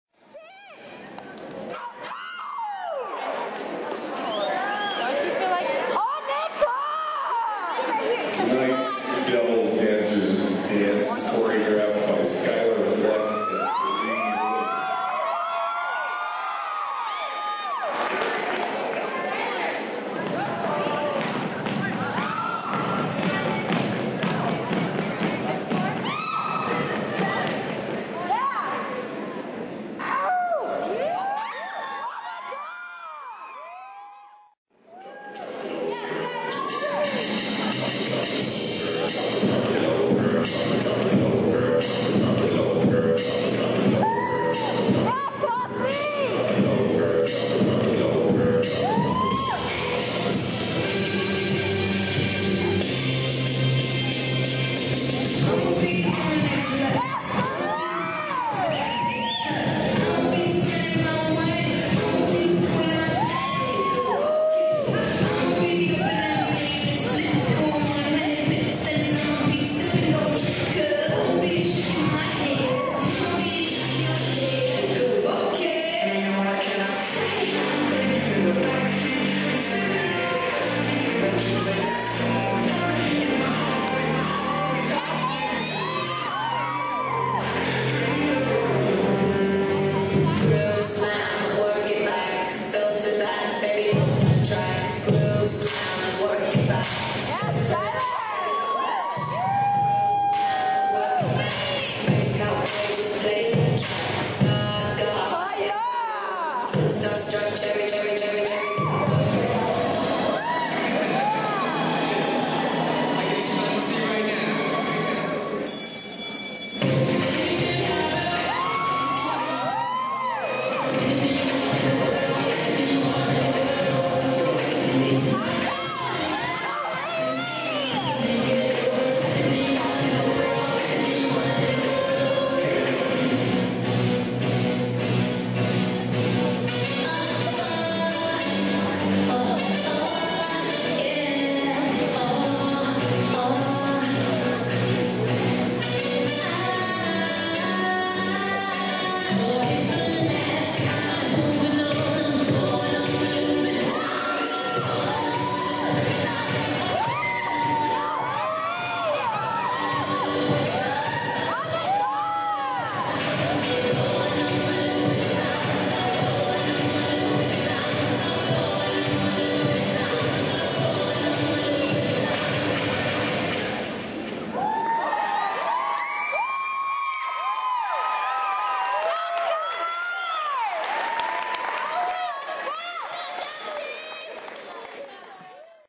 Eagle Valley HS Dance Team 2-2-10 performance
The Girls halftime performance at Basketball vs. Glenwood